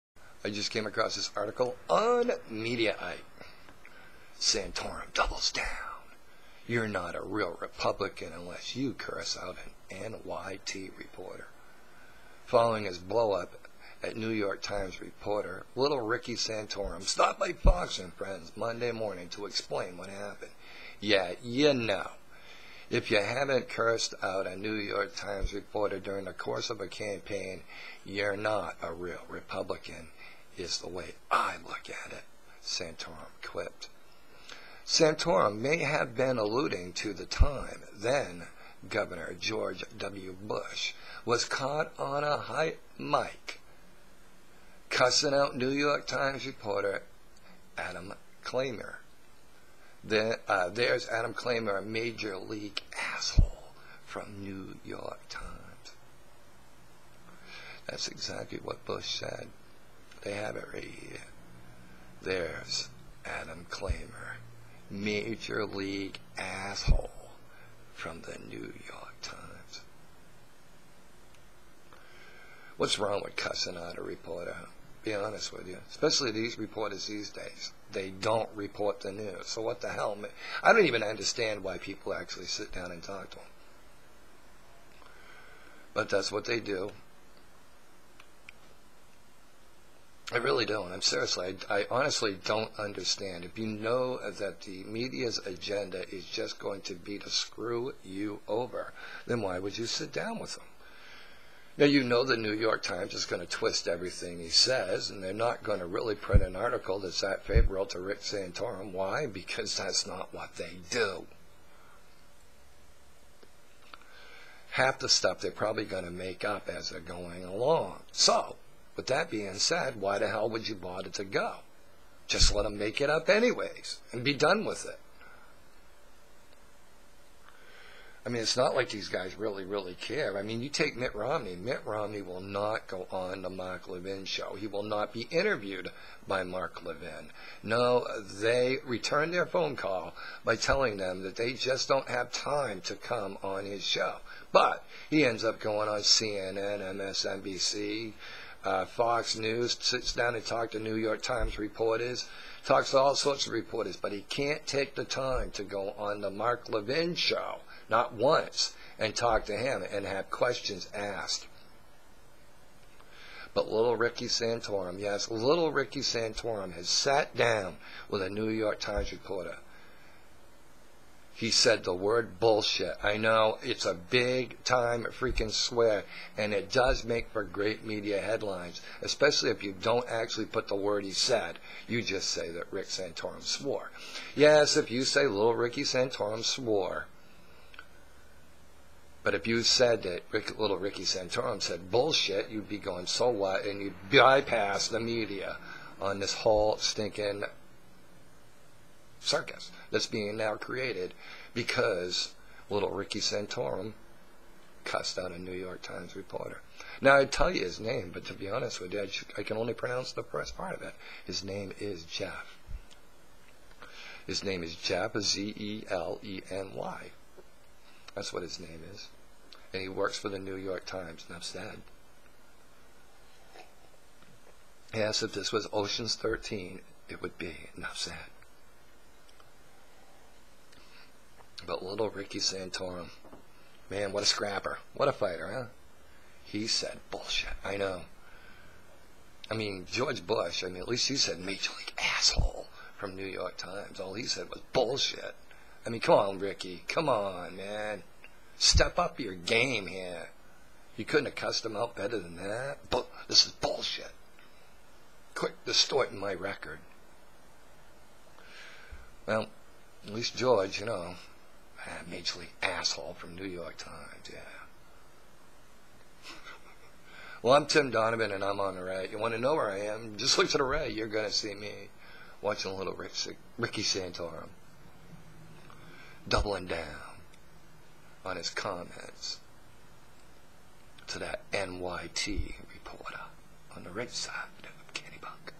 Little Ricky Santorum Is Doubling Down On New York Times Reporter
Santorum Doubles Down: 'You're Not A Real Republican Until You Curse Out A NYT Reporter'